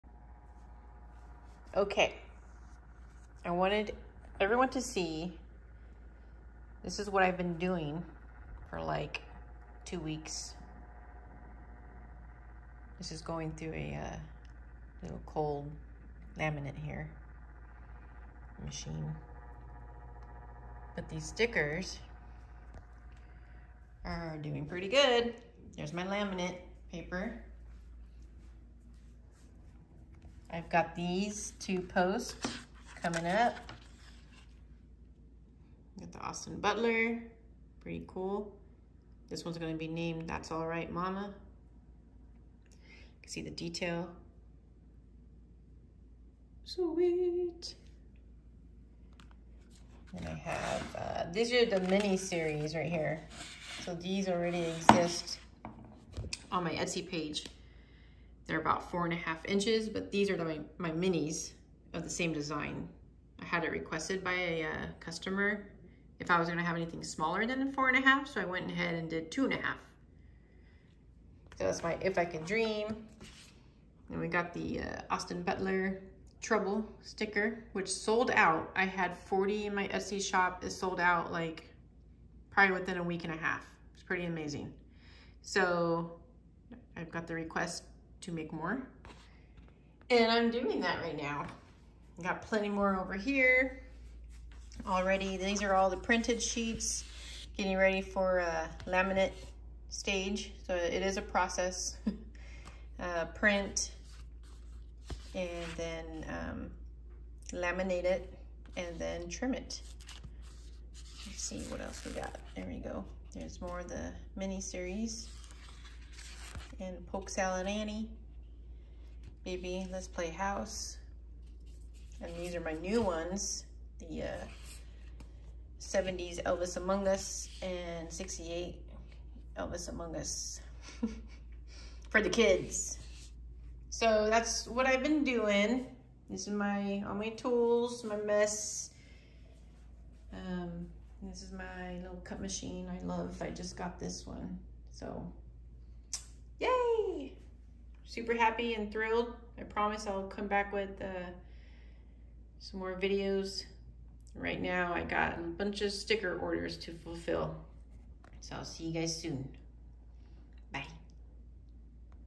Elvis Stickers in the Making!
What have I been DOING?! I know it's been a while since I've been able to sit at my computer and do an Elvis video, but this explains it all. Let me know what you think of the stickers! Thank you to those who've started a collection.